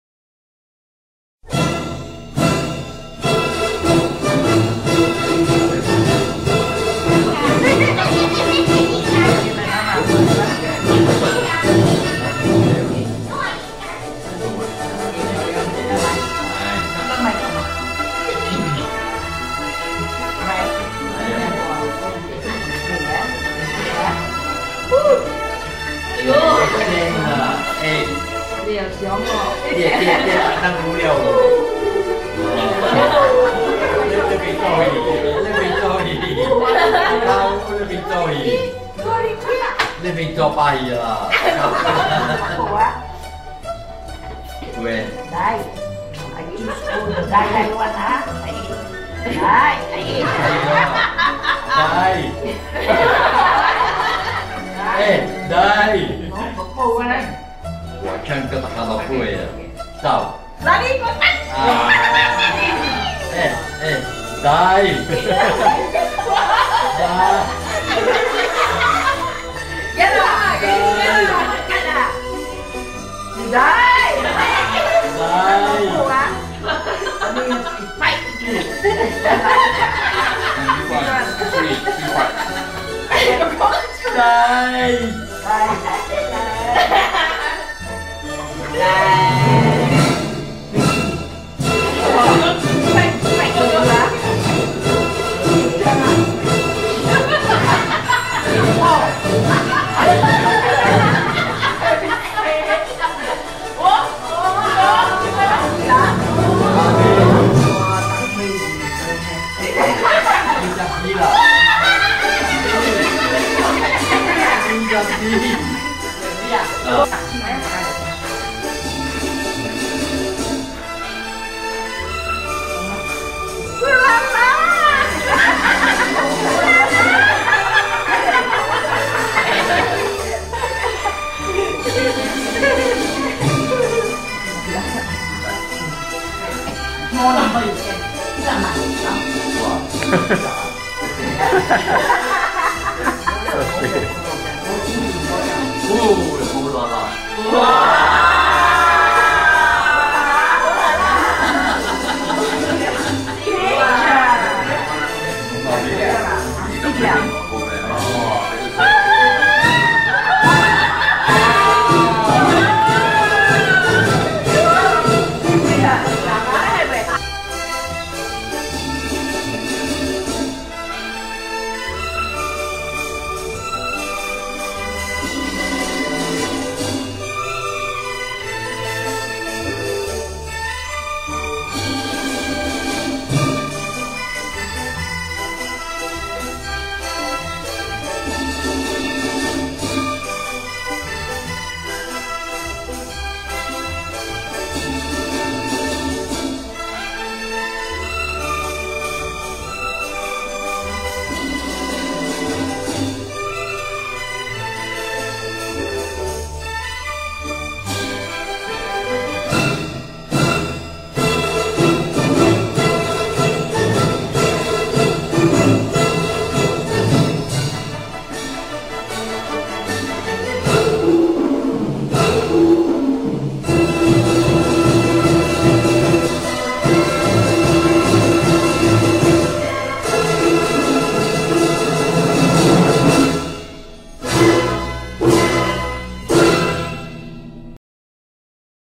Mother-in-law's new year 2013
Joyful moments with my mother-in-law during Chinese New Year celebrations